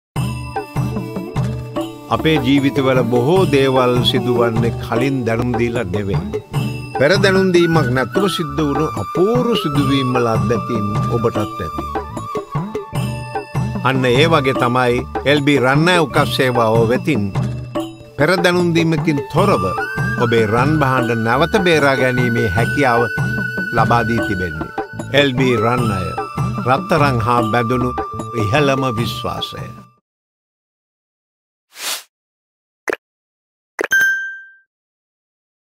LB Gold Loan | Redeem your gold without prior notice
රත්තරන් හා බැඳුණු ඉහළම විශ්වාසය රැදි LB Finance වෙතින් පෙර දැනුම් දීමකින් තොරව ඔබේ රන් භාණ්ඩ බේරාගන්න ඔබට පුළුවන්.

වැඩි විස්තර සඳහා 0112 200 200 අමතන්න